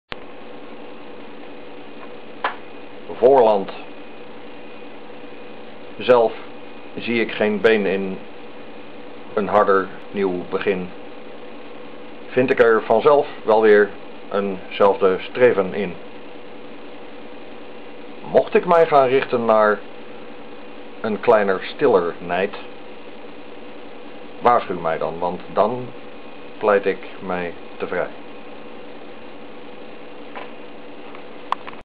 Chielie's Filmgedichten - Voorland
1 van de 35 gedichten uit de filmversie van 'Bundel' (ISBN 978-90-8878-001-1), door mijzelf voorgedragen. Ook op:
. Met dank aan Martijn Lucas Smit, voor het verzinnen van het originele belichtings-idee (toen met een tv, nu met een pc-monitor).

Nieuwe Toneelschuur, Haarlem.